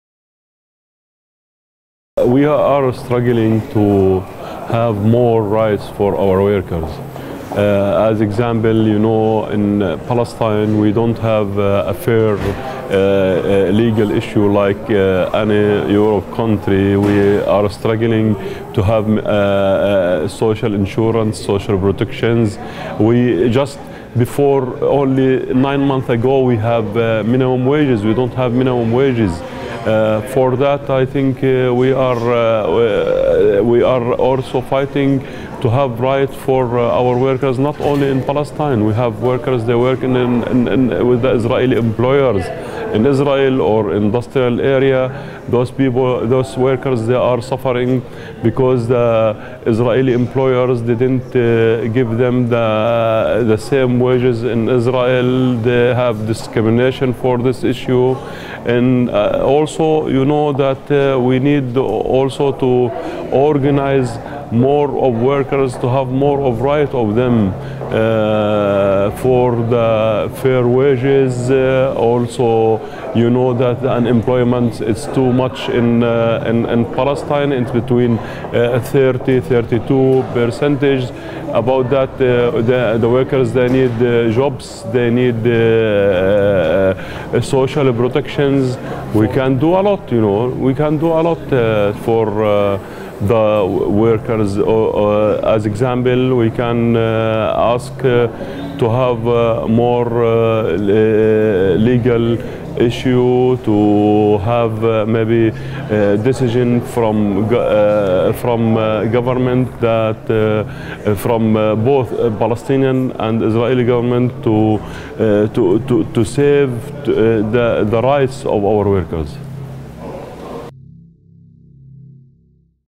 Shaher Saed, PGFTU
Managing the affairs of any trade union has its challenges, but Shaher Saed of the Palestine General Federation of Trade Unions has the most basic workers' rights issues.  There was just a recent success of getting workers a minimum wage in Palestine.  PGFTU not only stands for the rights of workers at home, but also the rights of Palestinians working in the neighboring nation.